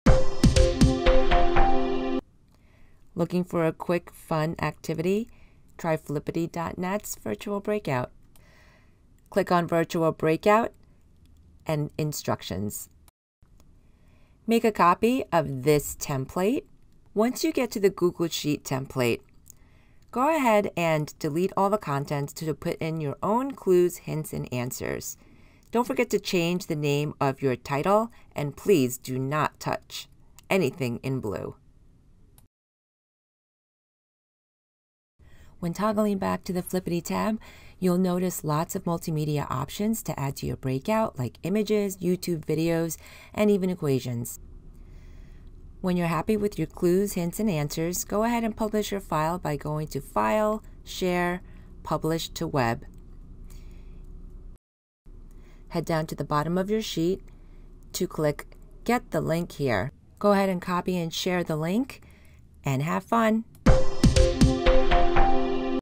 1 Minute Tech Tip- Create Virtual Breakout Rooms with Flippity
This 1-minute video shows how to create virtual breakout rooms using Flippity.